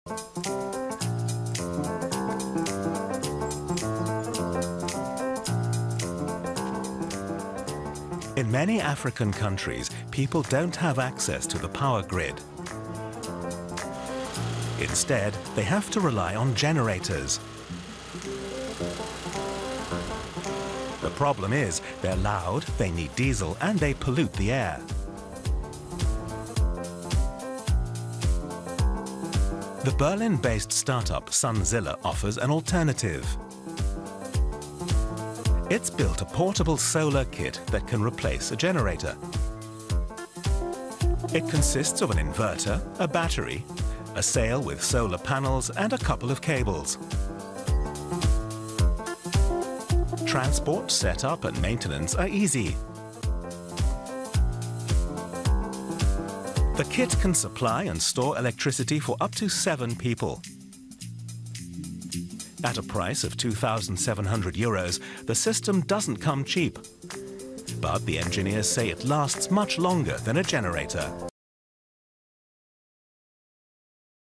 Making solar power portable | Eco-at-Africa
In many African countries people rely on dirty generators for power. Berlin start-up SunZilla offers an alterantive with its solar kits that can supply and store electricity for up to seven people.